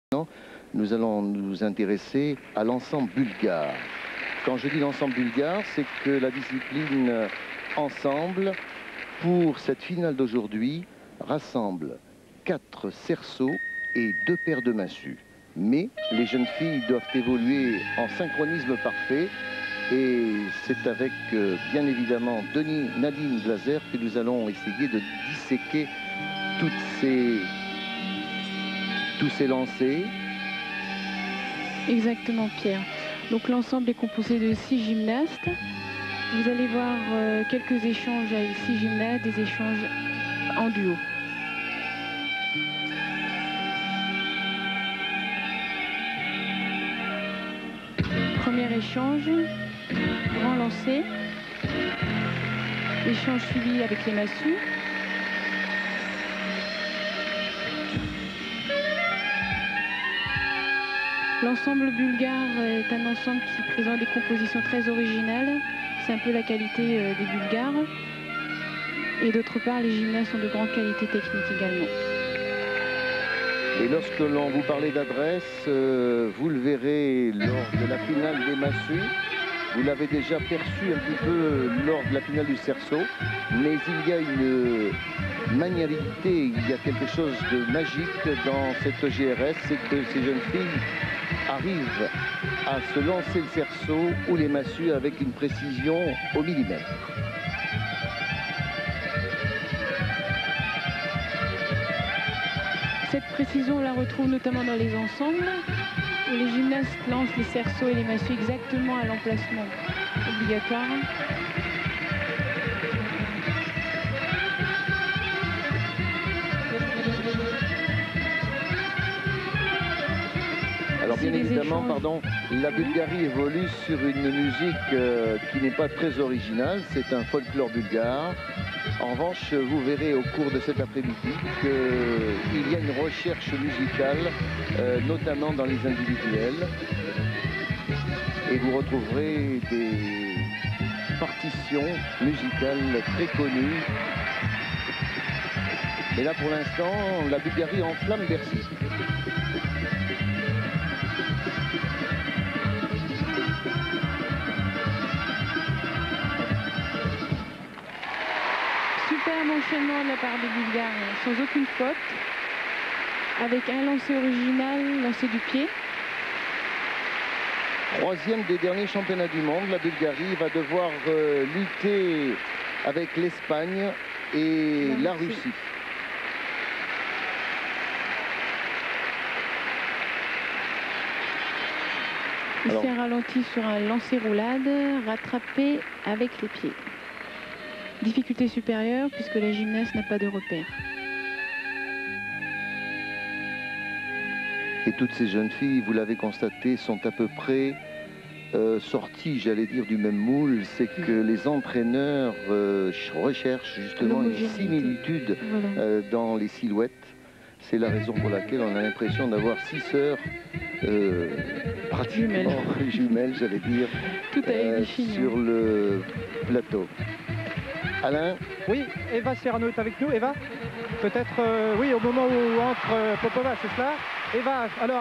BULGARIA 4 hoops 4 clubs - 1994 Paris worlds EF
BULGARIA (BUL) - 4 hoops 4 clubs final

'94 World championships in Paris

19.550
Rank : 1st